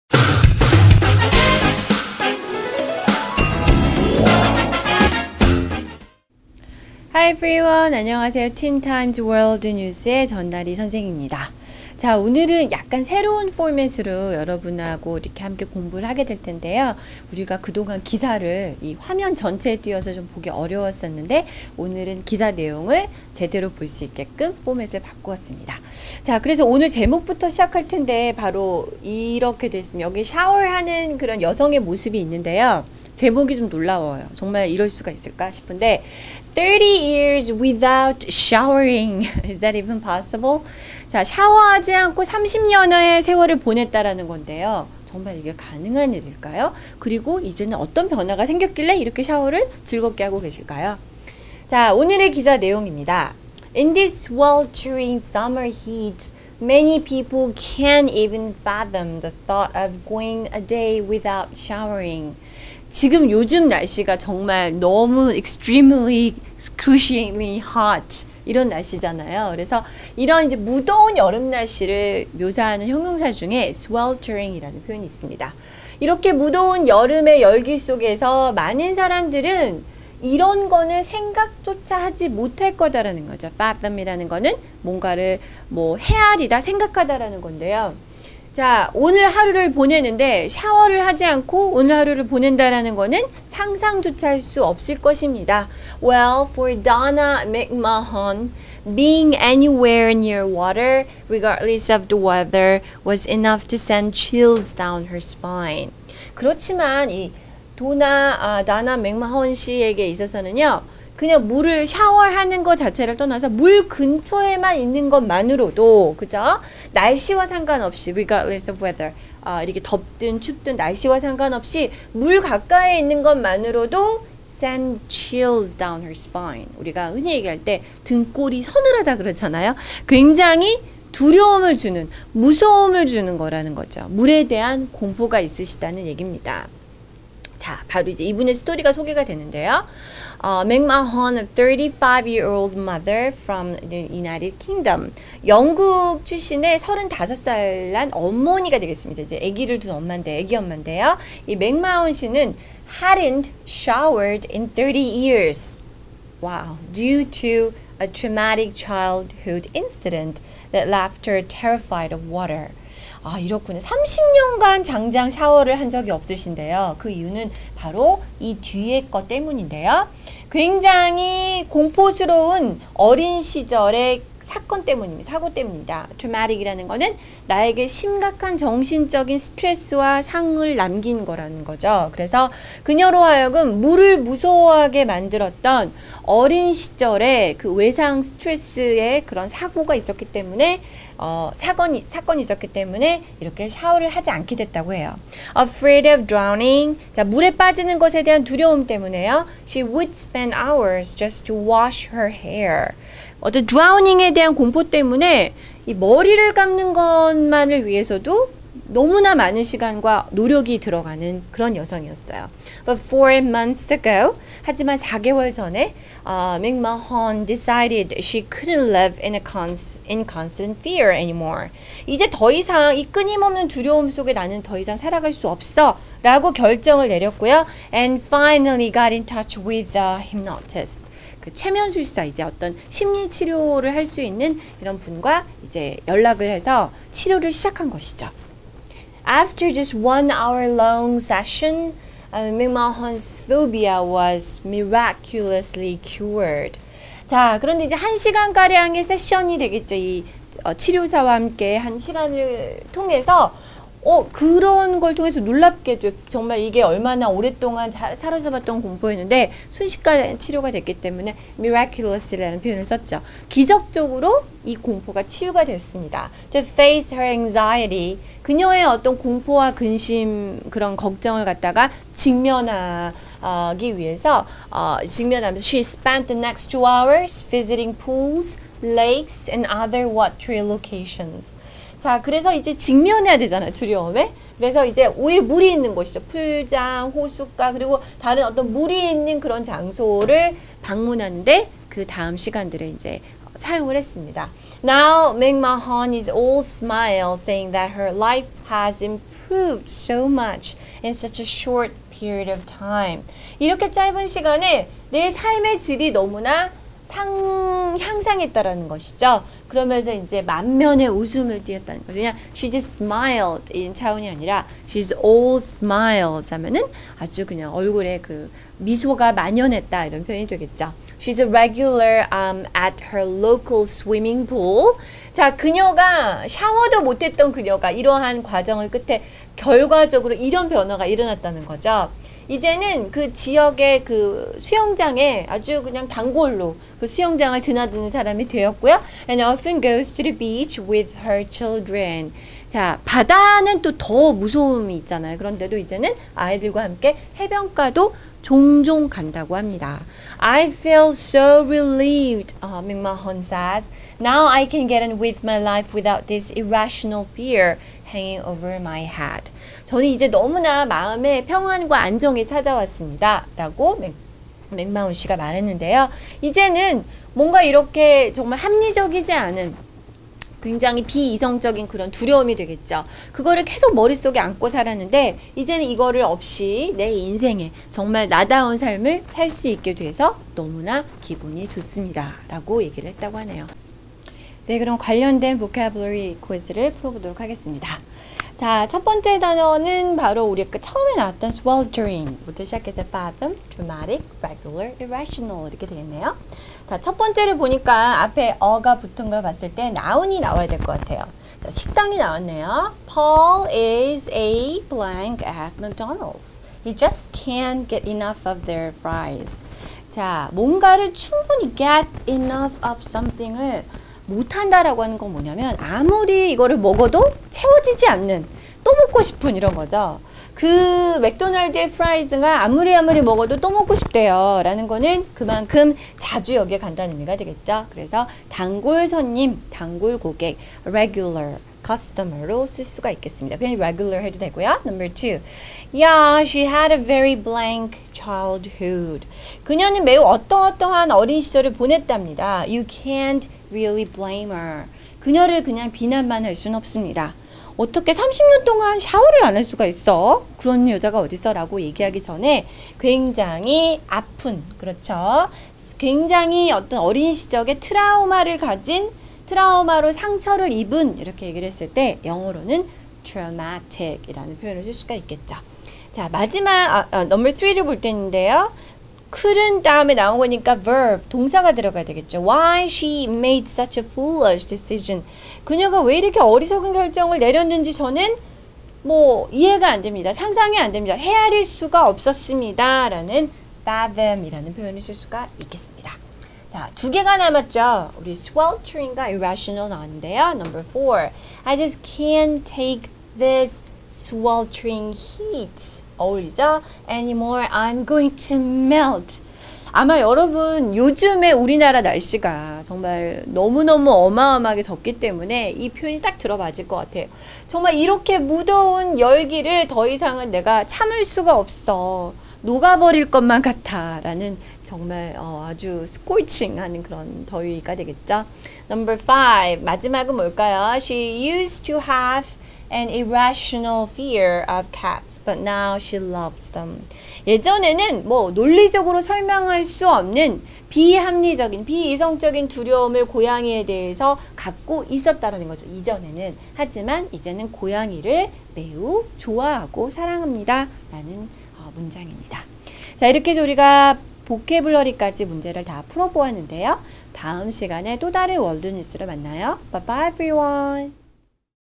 Thirty Years Without Showering - 전나리 선생님과 함께 하는 월드뉴스 동영상 강의 ( The Teen Times - No. 793 )
청소년 영자신문 틴타임즈 793호 월드뉴스 동영상 강의

30년 동안 씻지 않은 사람